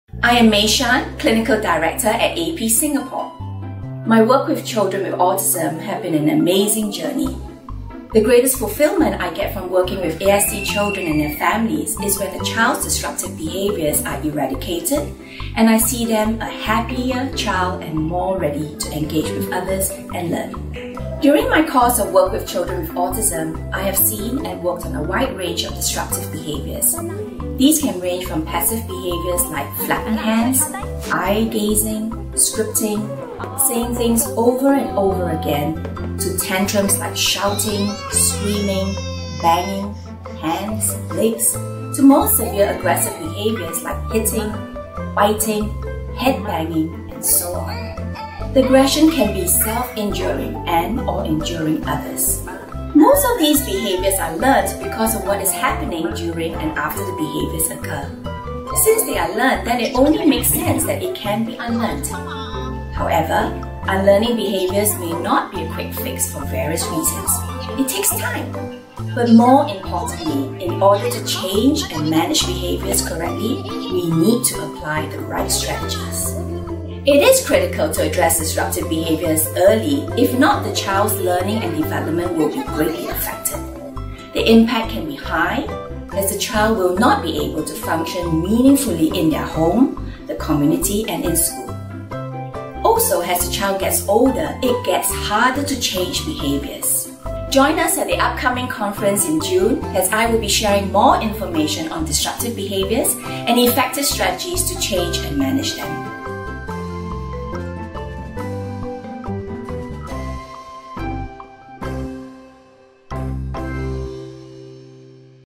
如何使用有效的ABA策略來管理滋擾性行為 (自閉症教學) Effective Strategies to Manage Disruptive Behaviors
今時今日，很多人對 自閉症 的了解還是存在很多誤解，特別是在亞洲地區。
首先，自閉症並不是家長養育不當造成的，而是先天形成的。另外，每年均有不少聲稱對治療自閉症患者有顯著功效的新療法、專業見解及建議、家長之間的口耳相傳，以及聲稱能治愈自閉症的藥物，但大部分都缺乏嚴謹的科學實証及測試以支持其聲稱的效用。

應用行為分析法 (ABA) 是迄今唯一得到最多研究支持及經科學証實對自閉症兒童最有效的療法。目前並無任何其他療法証實比ABA更為有效，或與ABA具同等的嚴謹科學實証。

在6月22日的深圳研討會，我們首次邀請到AP新加坡中心的治療總監黃美珊小姐，以及2位AP香港的資深治療顧問，為大家即場解答及細說如何有效應用ABA（應用行為分析）治療於不同程度的自閉症兒童。這次的講題包含了三大最受關注的問題：干擾性行為、社交興趣 及 社交技巧、自閉症兒童 的入學。首先，讓我們聽聽黃美珊小姐說說 干擾性行為。

Children with ASD may exhibit a wide range of Disruptive behaviours ranging from Passive behaviors to Aggressive behaviors. Most of these behaviours are learnt because of what is happening during and after the behaviours occur.

It is critical to address disruptive behaviours early if not the child’s learning and development will be greatly affected.

Join our conference in Shenzhen on 22 June, 2019 to learn more Effective Strategies to Manage Disruptive Behaviors!